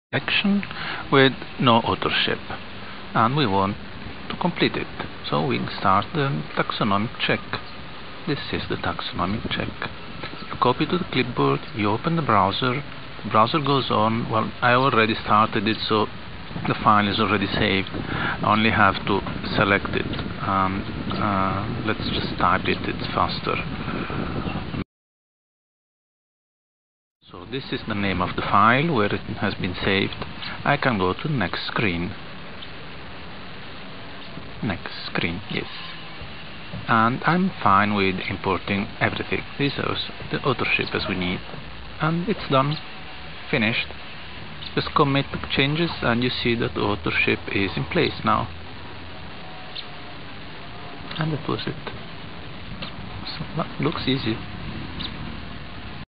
Bauble 004 (1.0.54) batch taxonomic check
Bauble's new Batch Taxonomic Check, or how to insert taxonomic authorship for all of your plant species and still keep time for your plants.
this is a first low res video